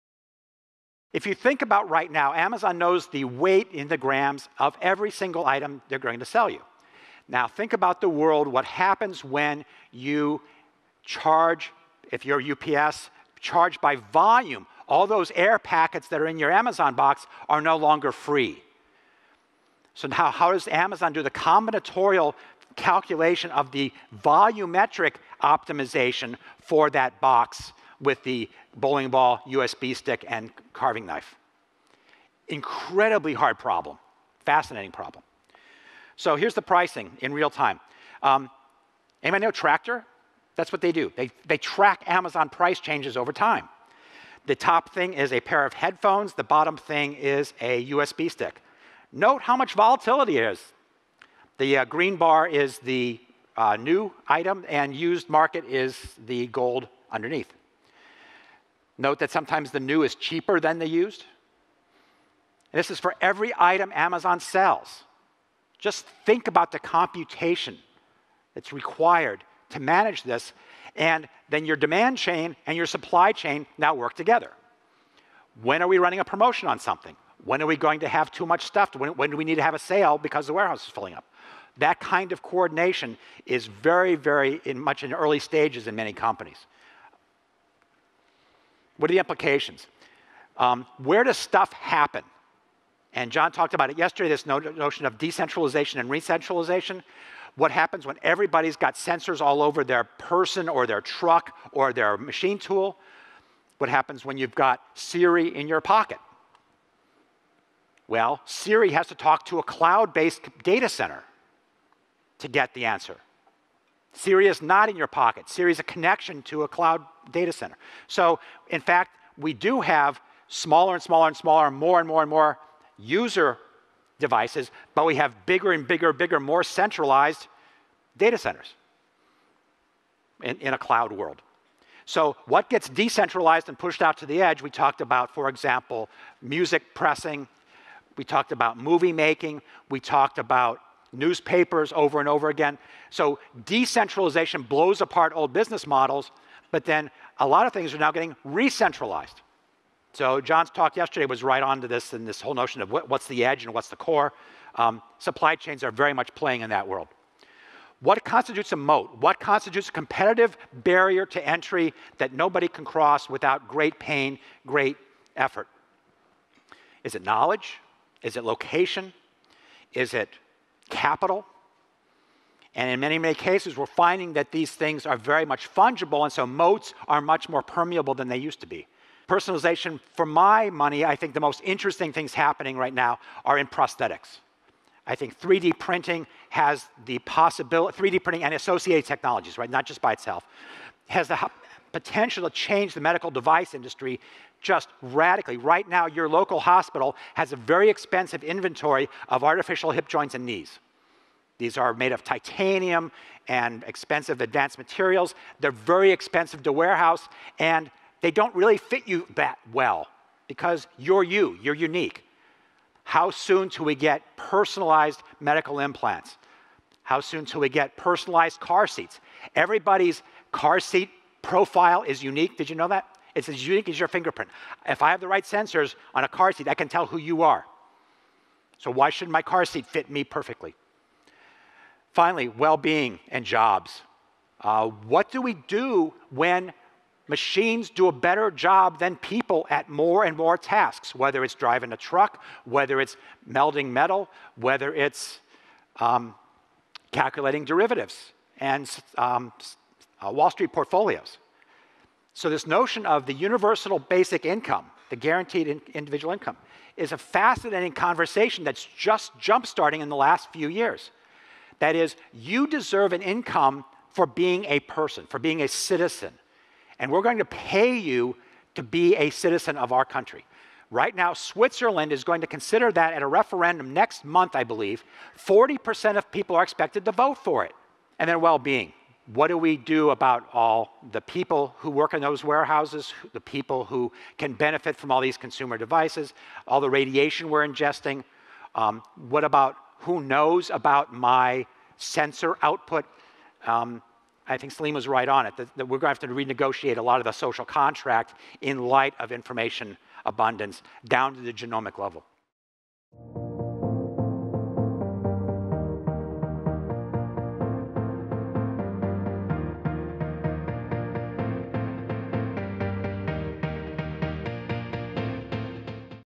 The Future of Supply Chain Management | Exponential Manufacturing
John Jordan and Supply Chain Management.